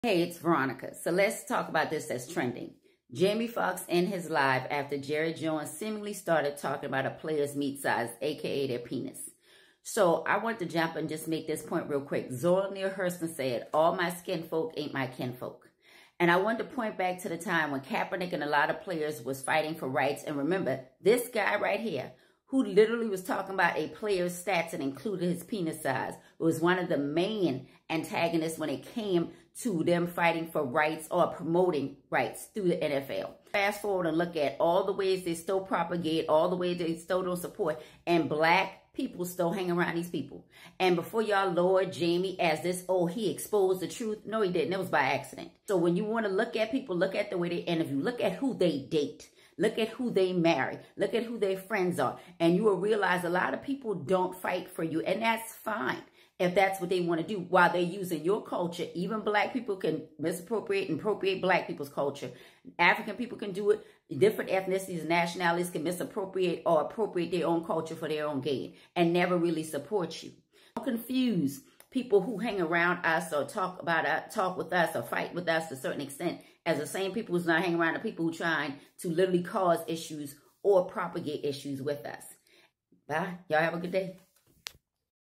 Jerry Jones, Jamie Foxx, and meat?
Dear black people, please do a little more vetting before inviting everyone to the cookout. Long time r*cist Jerry Jones is at it again in his latest stint on Instagram Live with Jamie Foxx. Veronica responds to this shocking story.